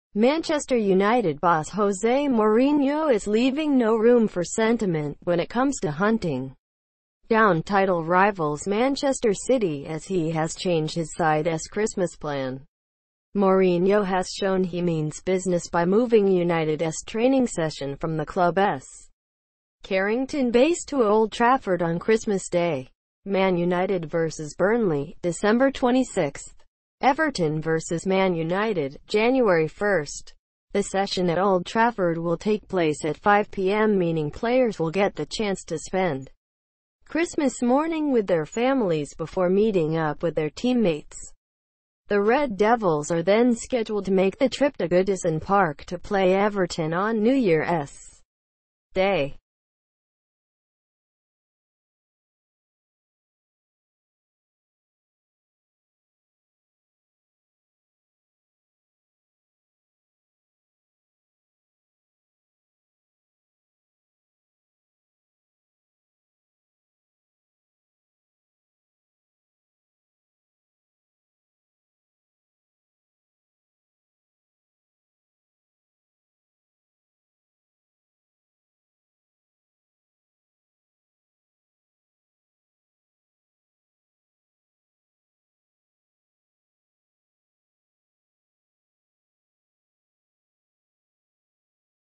Man Utd's Mourinho making 50 staff work on Christmas Day ‍
[ Manchester United boss Jose Mourinho is leaving no room for sentiment when it comes to hunting down title rivals Manchester City as he has changed his side s Christmas plan.  ,  Mourinho has shown he means business by moving United s training session from the club s Carrington base to Old Trafford on Christmas Day.  ,  Man United vs Burnley - Dec 26 ,  Everton vs Man United - Jan 1 ,  The session at Old Trafford will take place at 5pm meaning players will get the chance to spend Christmas morn